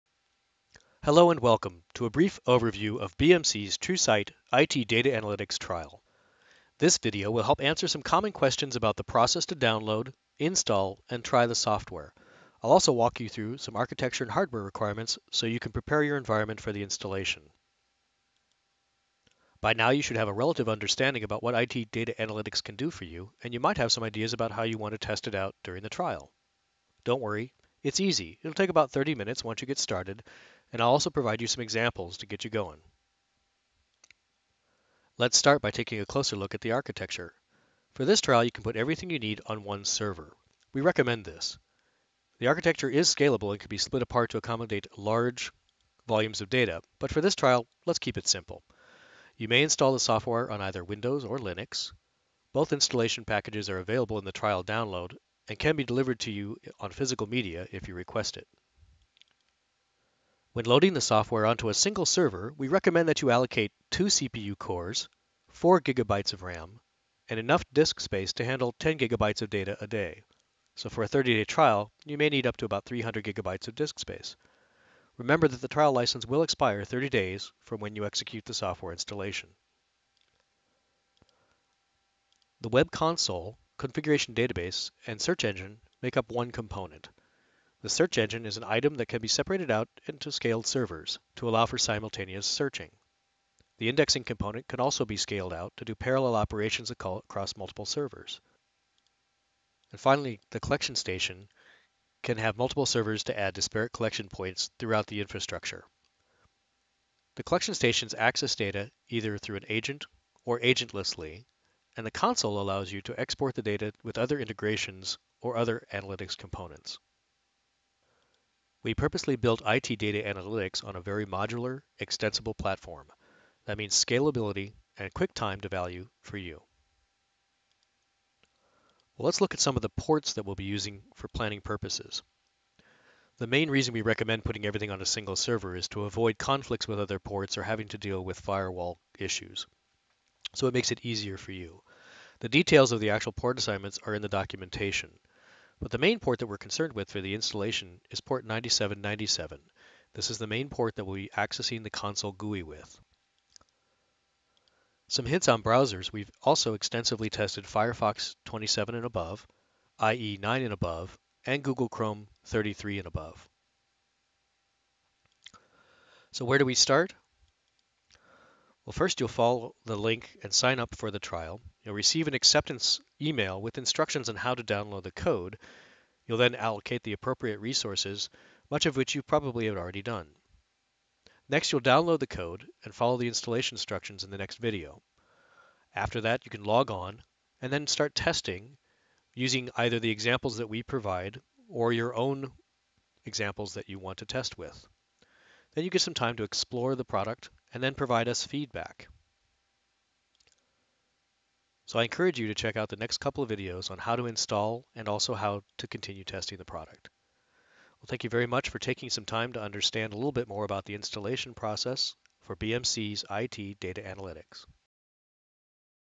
TrueSight IT Data Analytics: Architecture Overview
Check out this short 5 minute video explaining the architecture of TrueSight IT Data Analytics, installation options and configurations, recommended deployment techniques, and product licensing.